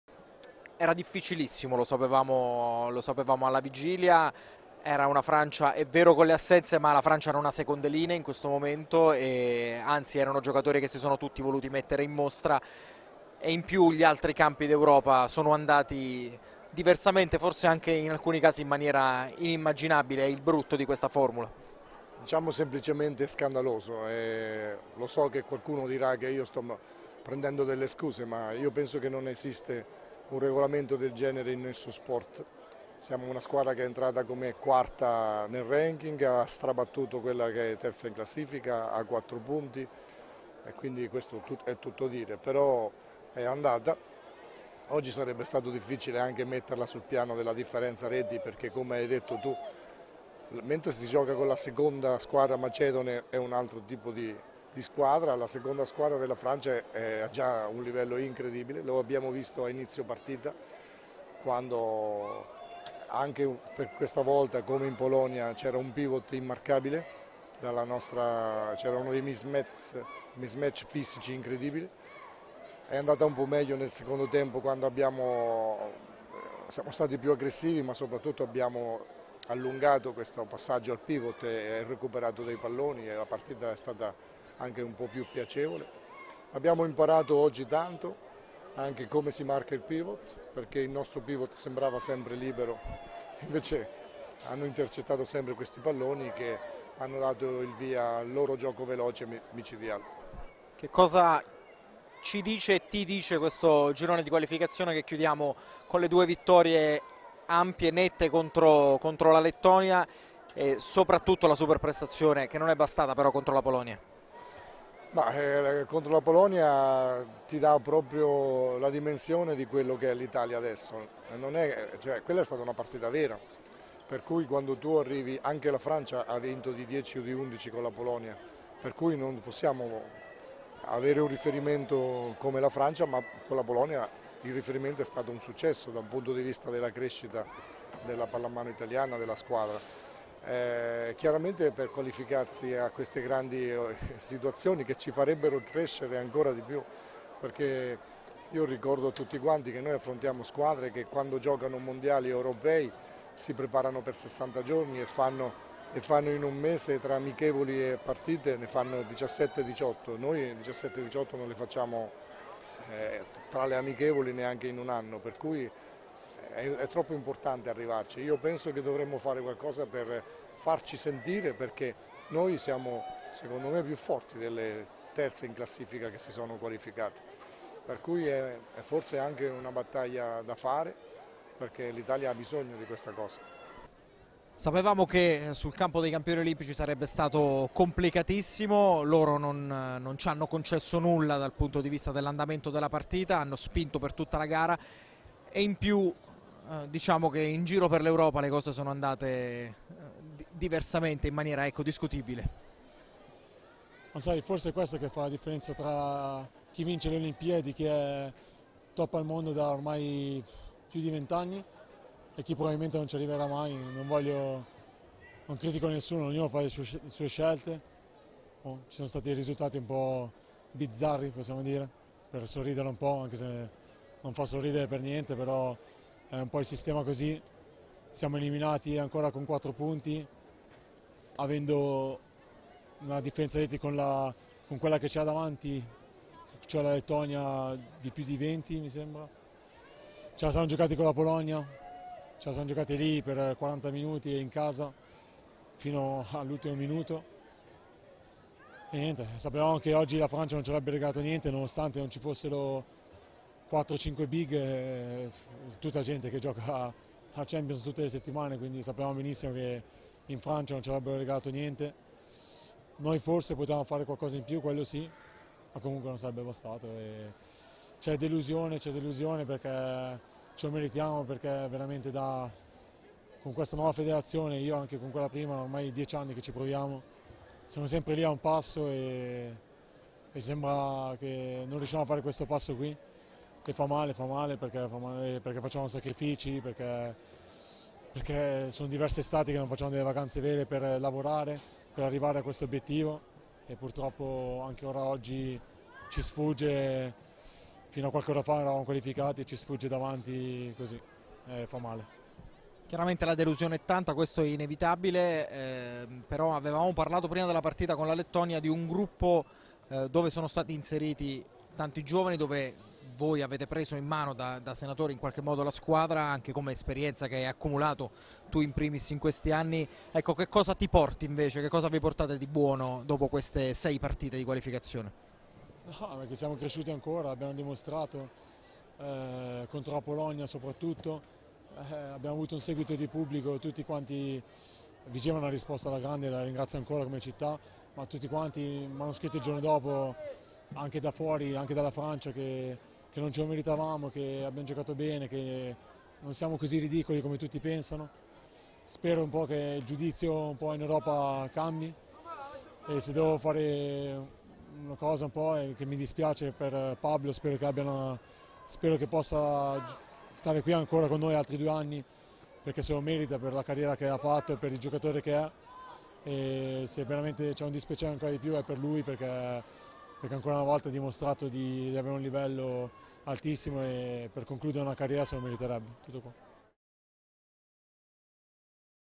azzurridentro | Francia - Italia 41-27 | Post-gara
🟦 Dalla Kindarena di Rouen, le voci del post-gara di 𝗙𝗥𝗔𝗡𝗖𝗜𝗔 - 𝗜𝗧𝗔𝗟𝗜𝗔 con il DT Riccardo Trillini e il capitano azzurro Andrea Parisini.